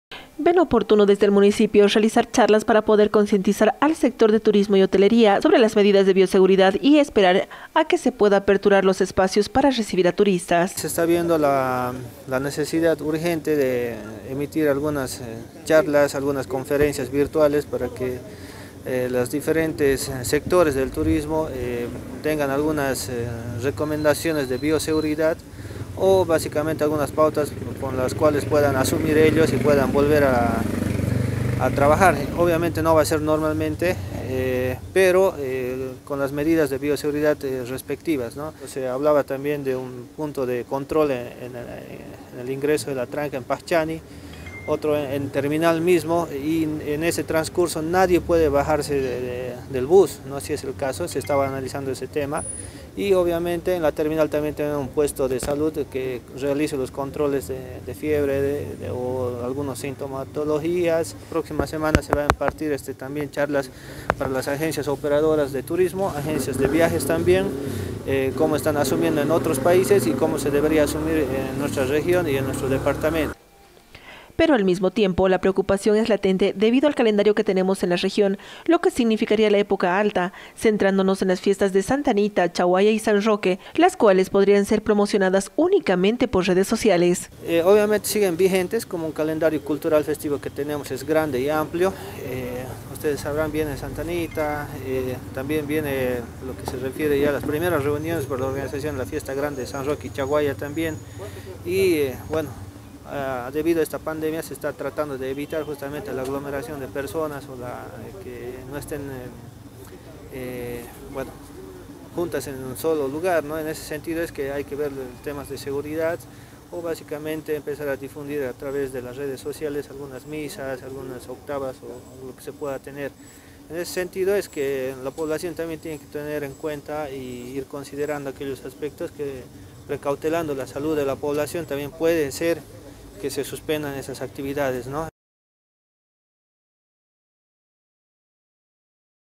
MUNICIPIO BRINDARÁ CHARLAS AL SECTOR HOTELERO Y DE TURISMO
El municipio ve oportuno brindar charlas para poder concientizar al sector de turismo y hotelería sobre medidas de bioseguridad y esperar a que se puedan reabrir los espacios para recibir a turistas.